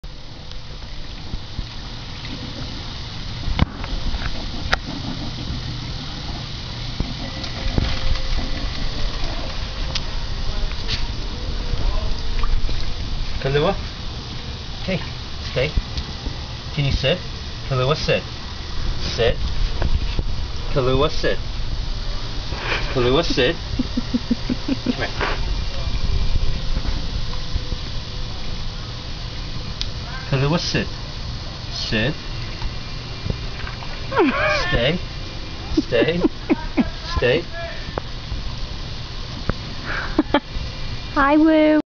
Kahlua and the Stream
We built a little stream that circulates the landscape in the back yard and Lu just loves it!  She loves to play in the water and any excuse to get dirty! She is now a 1yr old! Enjoy. Sorry it's so shaky.